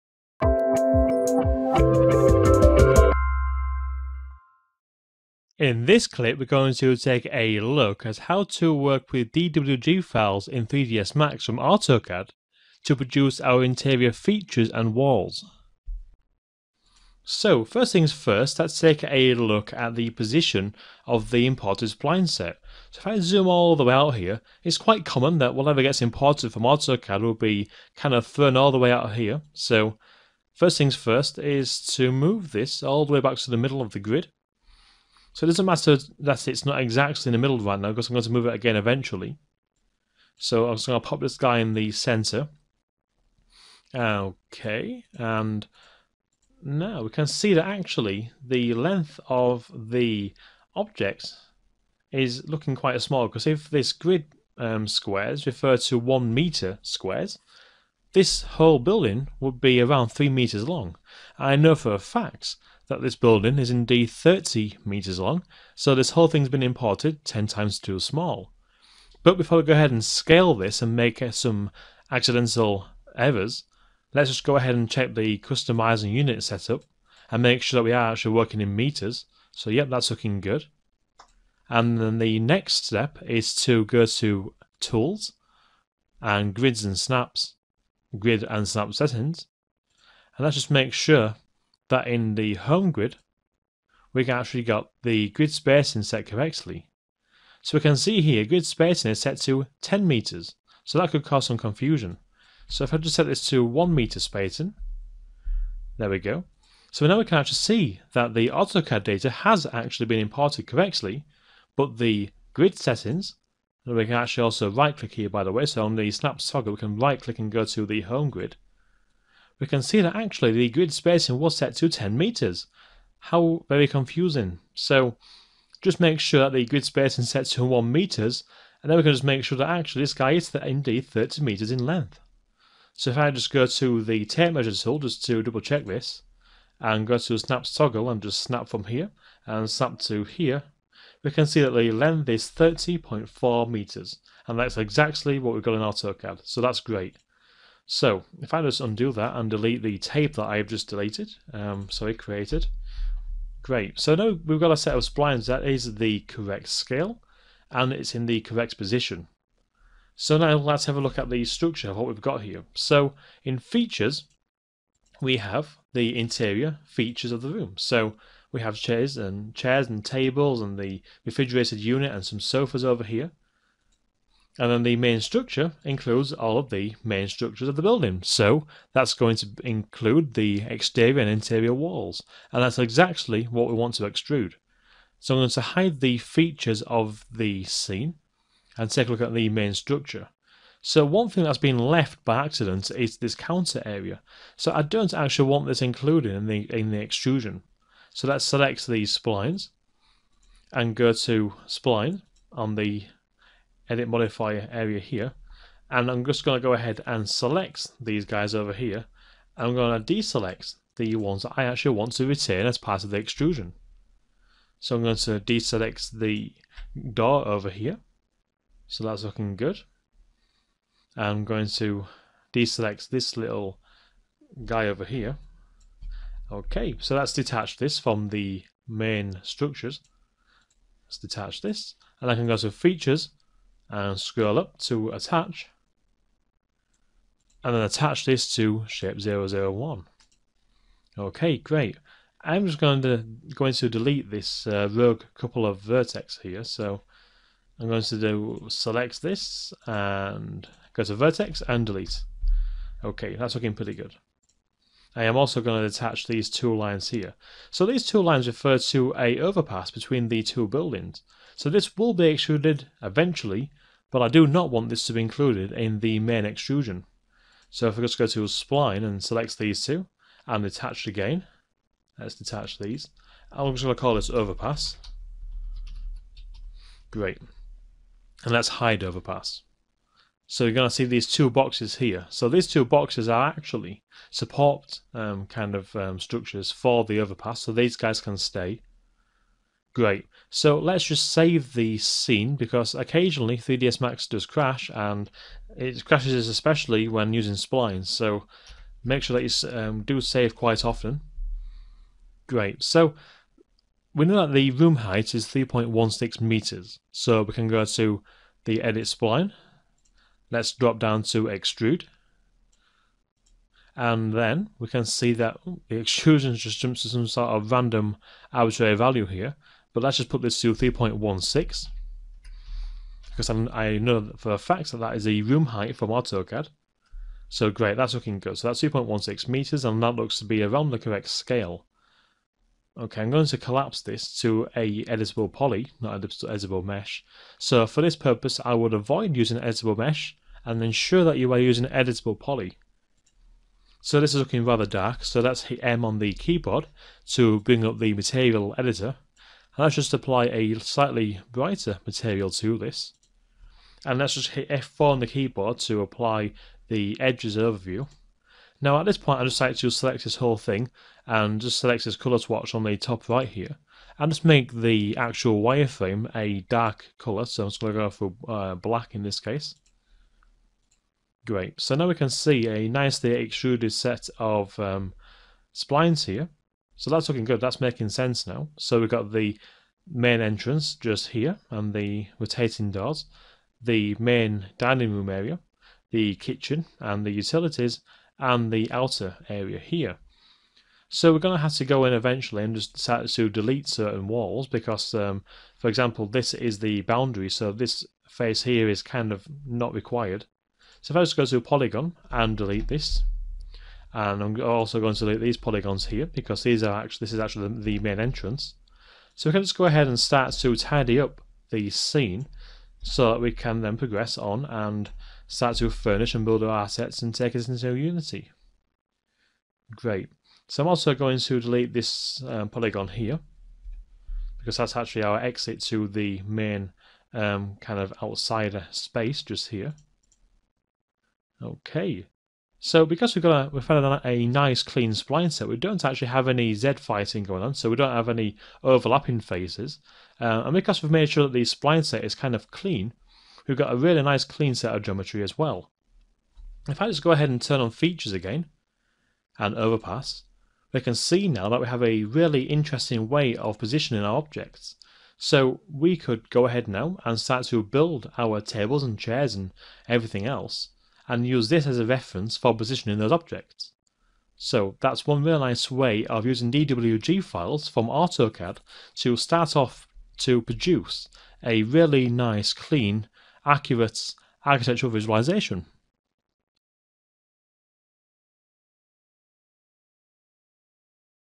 Using DWG Data from AutoCAD to Produce a Room Set
In this clip from Pluralsight’s “Creating Walk-throughs for Unity with Enlighten Using 3ds Max” course, we will learn how to bring DWG files from AutoCAD into 3ds Max in order to create the interior features of our virtual room.

5,000+ creative and tech training courses unlimited and online.